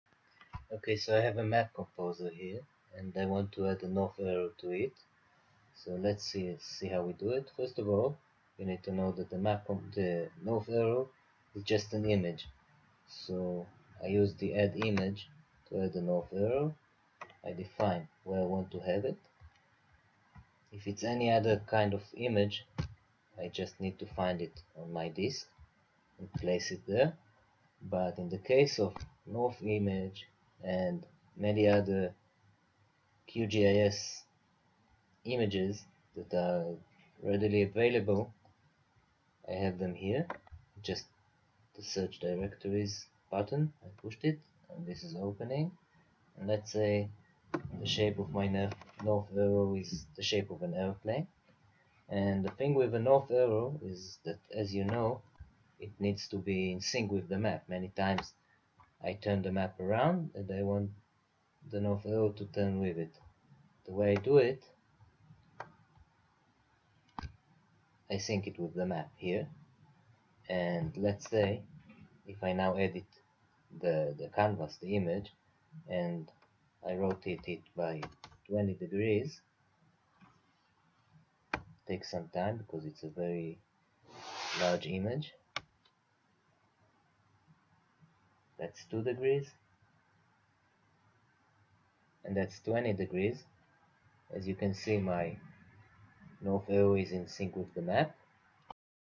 GIS 2015 07 map composer add north arrow
QGIS 2 add north arrow and sync with map in map composer
הוספת חץ צפון וסינכרון הכוון עם המפה